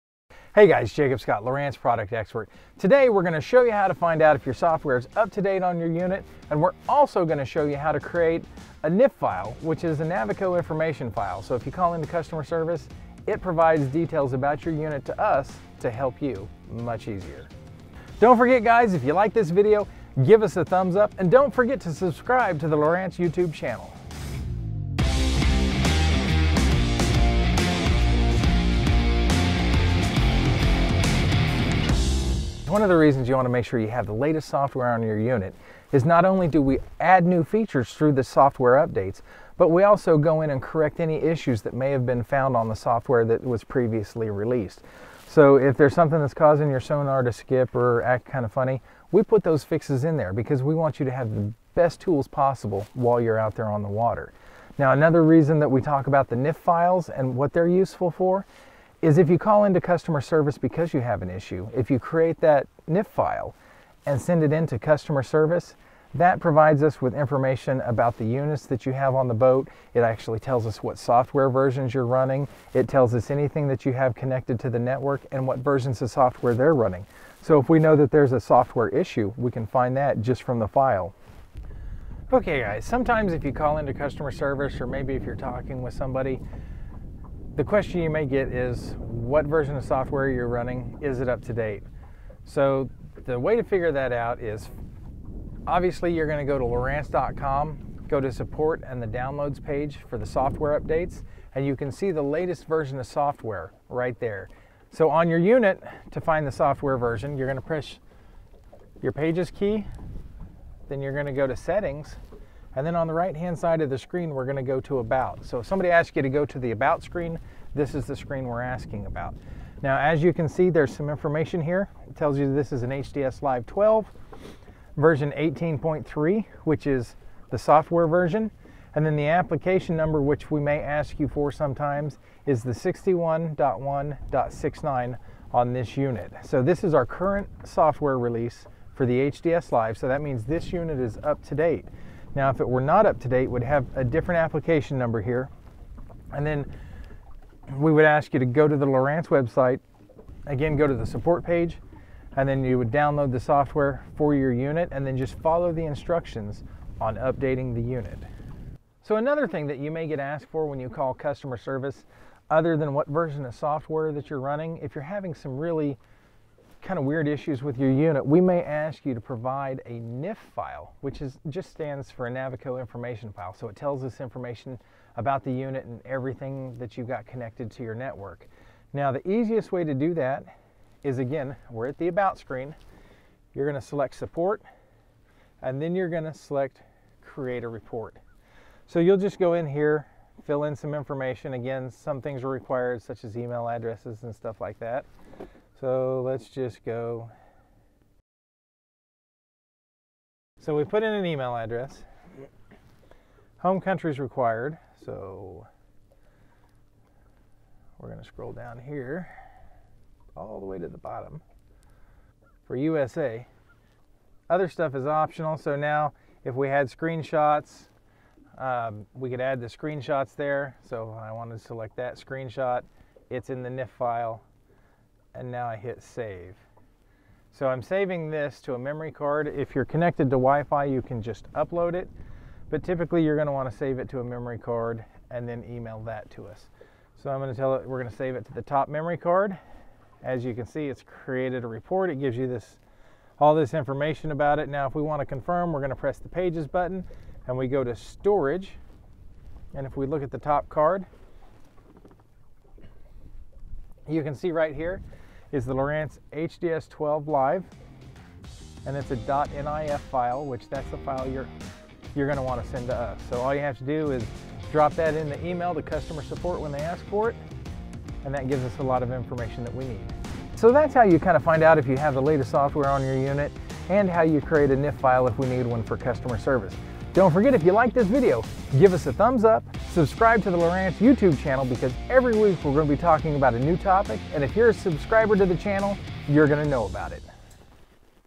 Getting the Latest Software & Creating NIF Files - HDS LIVE | Lowrance
Learn how to check the software version of your display unit and how to create a Navico Information File (NIF). NIF files include information about your display unit and your network, which make it much easier for our technical service team to assist you, if issues arise.